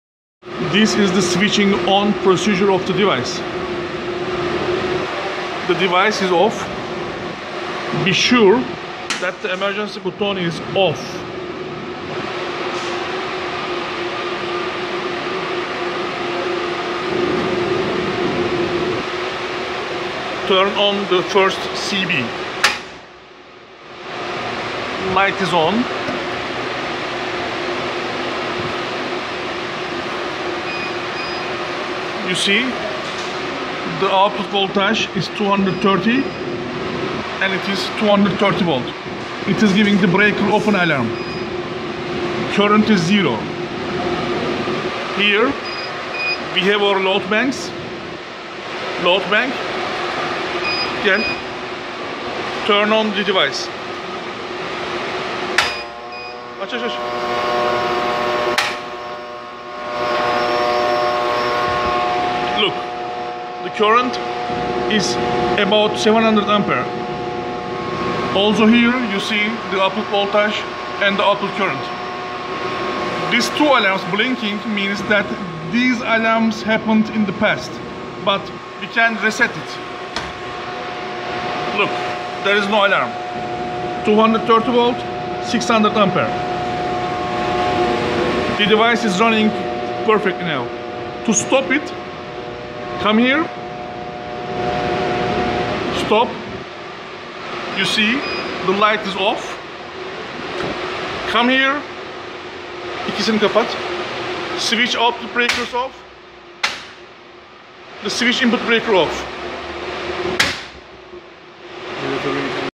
Rectifier 230V 720A Operation Video Marine Battery Supply And Charger
The rectifier is 230V DC output and 720Amper charger. There is two output and we have produced them for Colombia